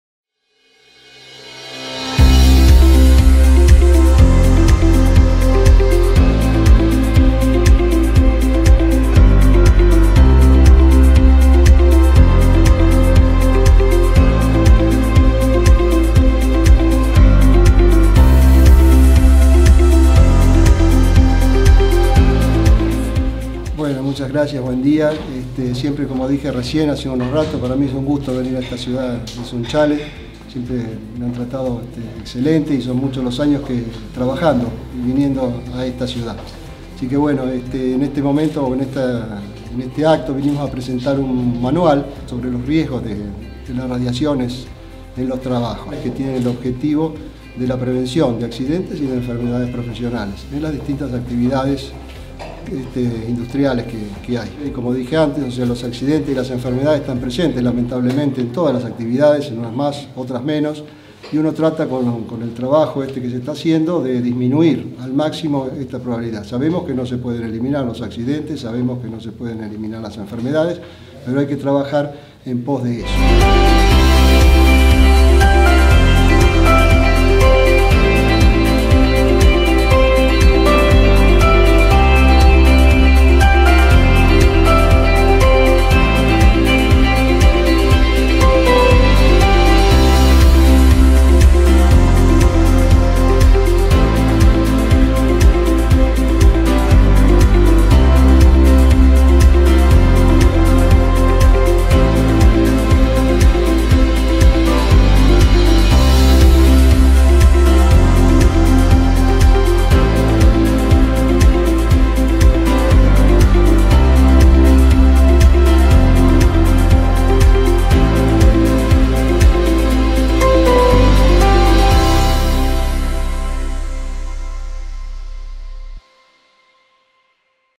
17 de Mayo: Taller Buenas Prácticas en Radiodiagnóstico
El jueves 17 de mayo, en el Centro de Educación Tecnológica de Atilra (Sunchales), se presentó el Manual de Prevención de Riesgos Laborales en Radiodiagnóstico elaborado por la Mesa de Radiología de la Superintendencia de Riesgos del Trabajo.
Tras la presentación de los contenidos del Manual y las palabras de apertura del Subgerente de Desarrollo en Salud y Seguridad de la SRT, Dr. Eduardo Barrón, se realizó un taller de identificación de riesgos y elaboración de buenas prácticas del que participaron 120 técnicos, licenciados y estudiantes de radiología de la región centro (Santa Fe, Rosario y Córdoba).